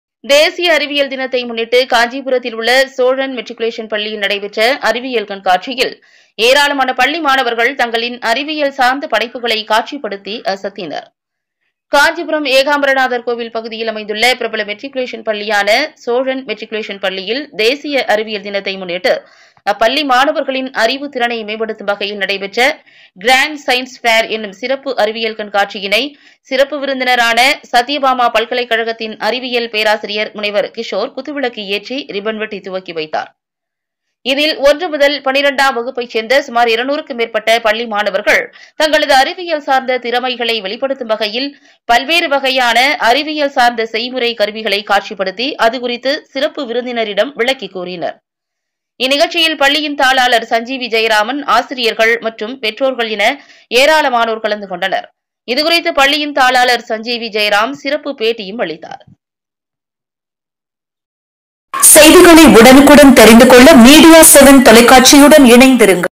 காஞ்சிபுரம் சோழன் மெட்ரிகுலேஷன் பள்ளியில் அறிவியல் கண்காட்சி...
தேசிய அறிவியல் தினத்தை  முன்னிட்டு காஞ்சிபுரத்திலுள்ள சோழன் மெட்ரிகுலேஷன் பள்ளியில் நடந்த அறிவியல் கண்காட்சியில்  ஏராளமான பள்ளி மாணவர்கள் தங்களின் அறிவியல் சார்ந்த படைப்புகளை காட்சிப்படுத்தி அசத்தினர்.

காஞ்சிபுரம்  ஏகாம்பரநாதர் கோவில்  பகுதியில் அமைந்துள்ள பிரபல மெட்ரிகுலேஷன் பள்ளியான சோழன் மெட்ரிக்குலேஷன் பள்ளியில் தேசிய அறிவியல் தினத்தை முன்னிட்டு,அப்பள்ளி மாணவர்களின் அறிவுத்திறனை மேம்படுத்தும் வகையில் இன்று நடைபெற்ற GRAND SCIENCE FAIR எனும்  சிறப்பு அறிவியல் கண்காட்சியினை, சிறப்பு விருந்தினரான சத்தியபாமா பல்கலைக்கழகத்தின் அறிவியல் பேராசிரியர் முனைவர் கிஷோர் குத்து விளக்கு ஏற்றி, ரிப்பன் வெட்டி  துவக்கி வைத்தார்.

இதில் ஒன்று முதல் 12வகுப்பை சேர்ந்த சுமார் 200க்கும் மேற்பட்ட பள்ளி மாணவர்கள்
பள்ளி மாணவர்கள் தங்களது  அறிவியல் சார்ந்த திறமைகளை வெளிப்படுத்தும்  வகையில்  பல்வேறு வகையான அறிவியல் சார்ந்த செய்முறை கருவிகளை காட்சிப்படுத்தி அது குறித்து  சிறப்பு விருந்தினரிடம் விளக்கிக் கூறினர்.

இந்நிகழ்ச்சியில் பள்ளியின் தாளாளர் சஞ்சீவி ஜெயராமன், ஆசிரியர்கள் மற்றும் பெற்றோர்கள் என ஏராளமானோர் கலந்துக்கொண்டனர். இதுகுறித்து பள்ளியின் தாளாளர் திரு.சஞ்சீவி ஜெயராம் சிறப்பு பேட்டியும் அளித்தார்